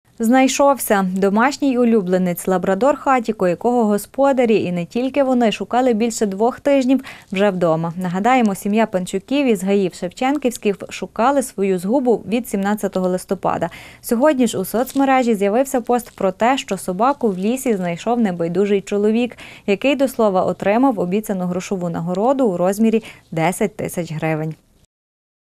Знайшовся лабрадор Хатіко, якого шукали більше 2-х тижнів
Знайшовся. Домашній улюбленець - лабрадор Хатіко, якого господарі, і не тільки вони, шукали більше 2-х тижнів вже вдома. Нагадаємо, сім'я Панчуків із Гаїв-Шевченківських шукали свою згубу від 17-го листопада. Сьогодні ж у соцмережі з'явився пост про те, що собаку в лісі знайшов небайдужий чоловік, який, до слова. отримав обіцяну грошову нагороду у розмірі 10 000 гривень.